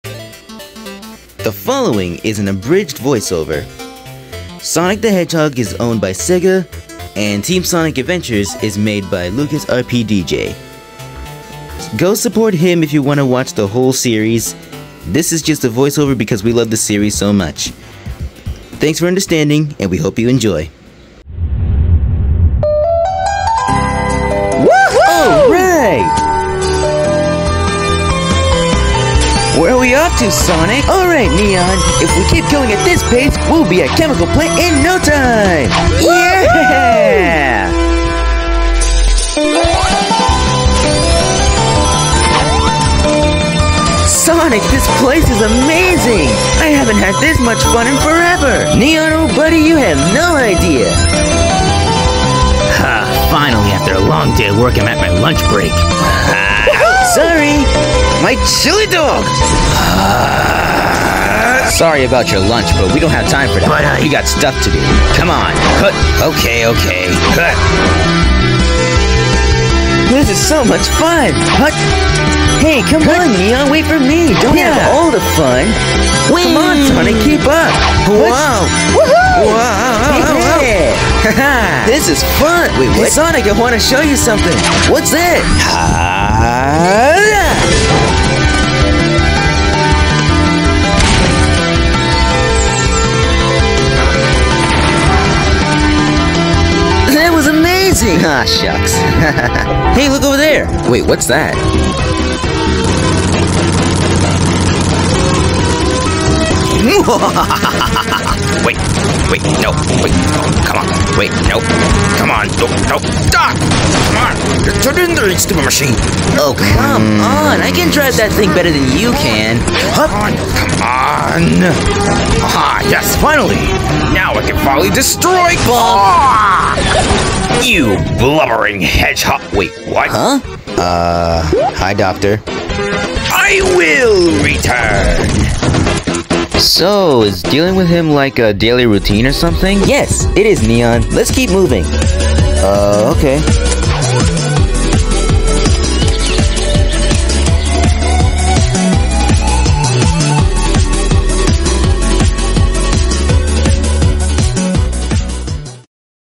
Team Sonic Abridged act 2: Sonic and Neon
Neon doesn't have much to say so coming up with a good voice for him and Sabre and maybe the sonic clones will be pretty tough but I'll manage over time and like I said I love this series and that's why I'm voicing over scenes I like most as best I can.

animation owner: @LucasRPDJ